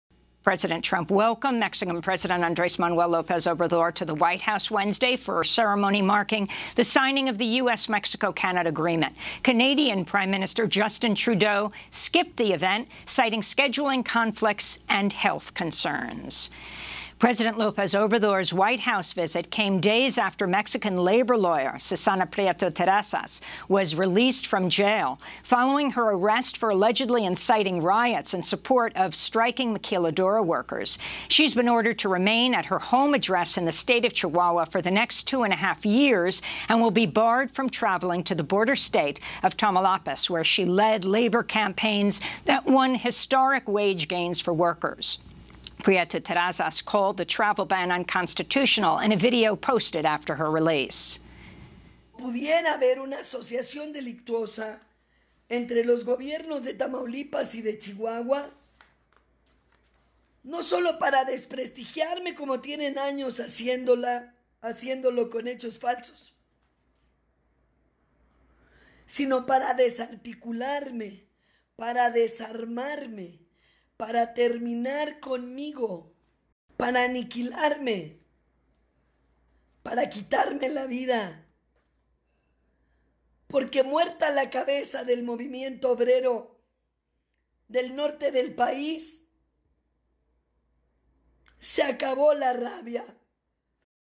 Liberan de la cárcel a líder laboral mexicana, pero se le prohíbe visitar a los maquiladores
La abogada mexicana Susana Prieto Terrazas denuncia que podría haber “una asociación delictuosa” en su contra. “No solo para desprestigiarme, como tienen años haciéndolo con hechos falsos, sino para desarticularme, para desarmarme, para terminar conmigo, para aniquilarme, para quitarme la vida, porque muerta la cabeza del movimiento obrero del norte del país, se acabó la rabia”, advierte.

Prieto Terrazas fue liberada de la cárcel tras haber sido arrestada por una supuesta incitación a los disturbios en apoyo de los trabajadores de las maquiladoras en huelga.